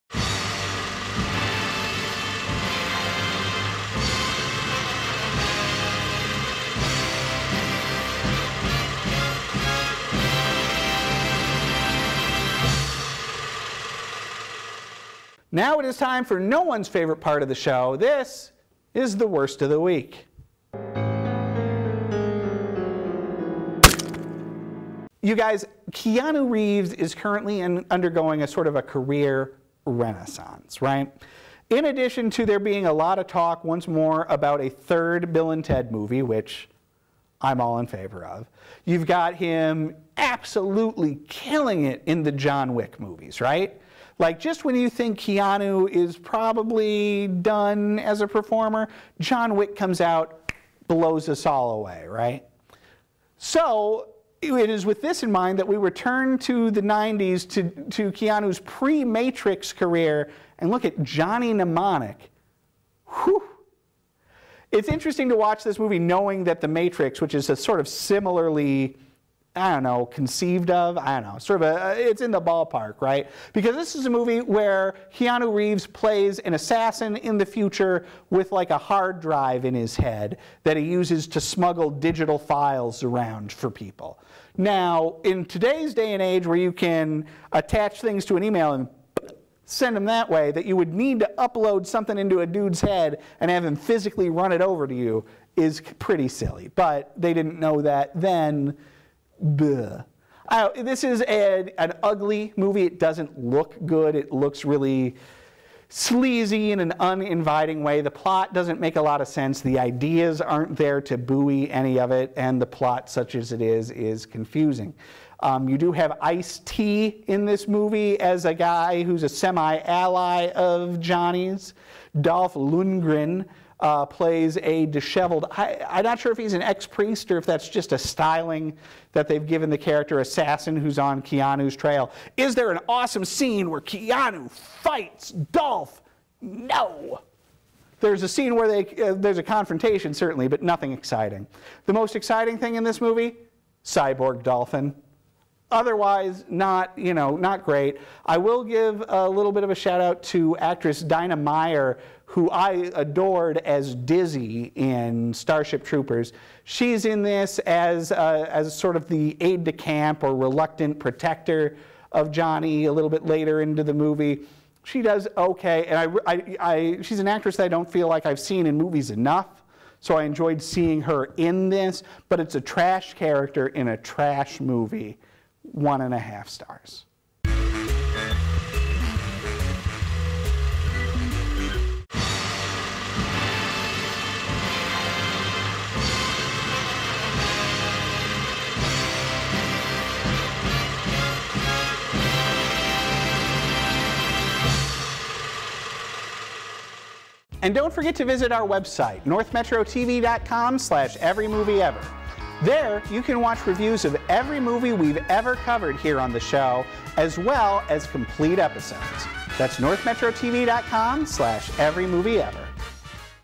Every Movie Ever - Johnny Mnemonic
We're reviewing every movie ever made. Today, Johnny Mnemonic, essentially The Matrix, but terrible.